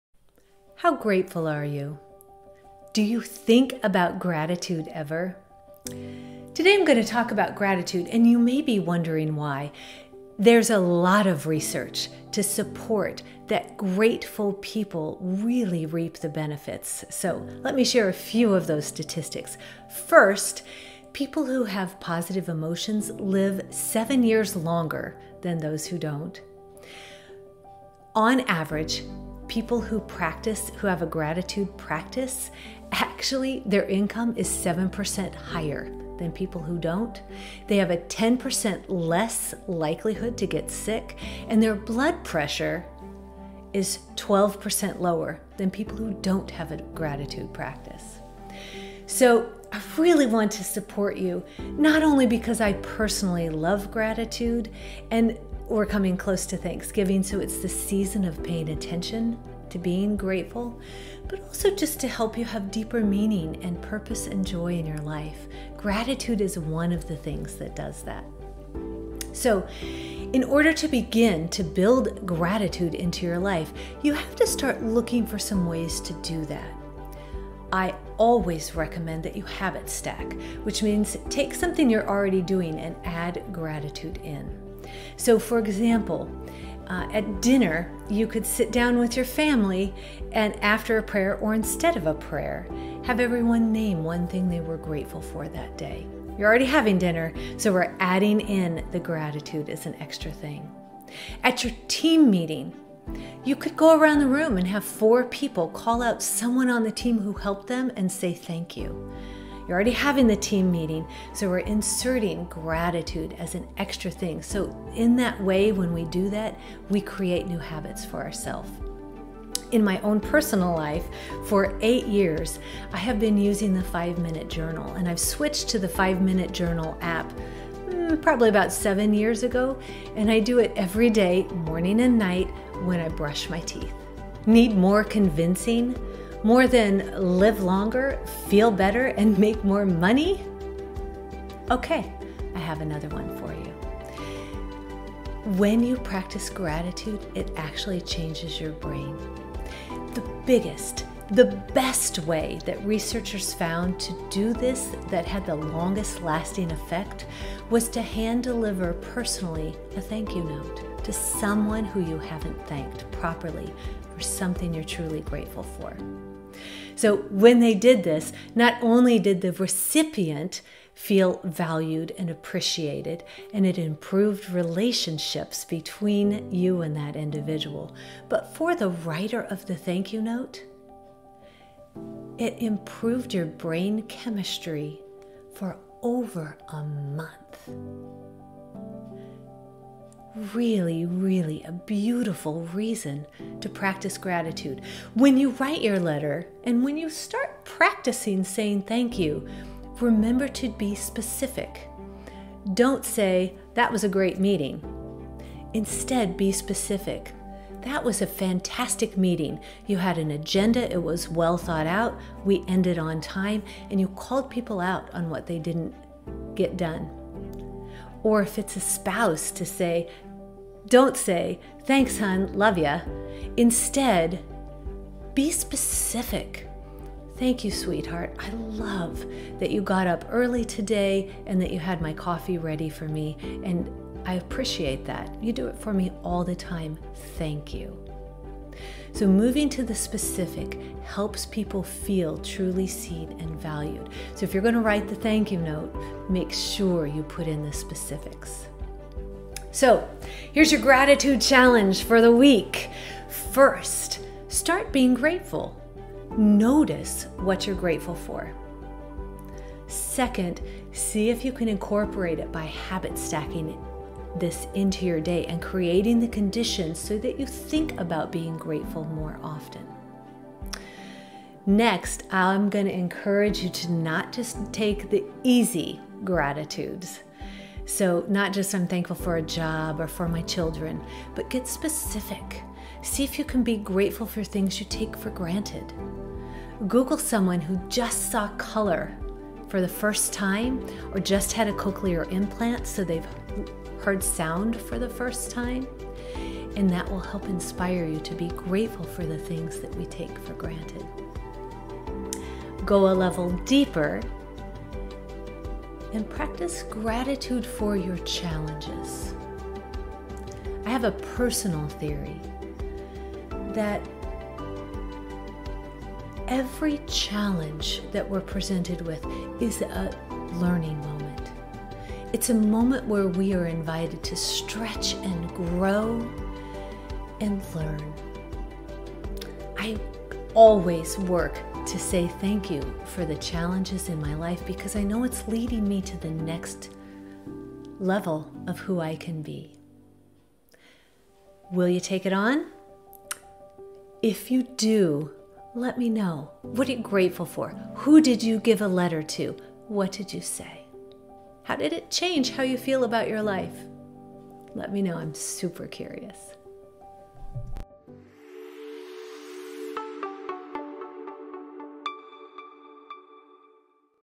Gratitude
How grateful are you? Do you ever think about gratitude? There's significant research to support the benefits grateful people reap.

In today's video, Sarah Fruehling from Fruehling Coaching and Facilitation explains how to harness the power of gratitude, for a positive impact on our health, happiness, and success.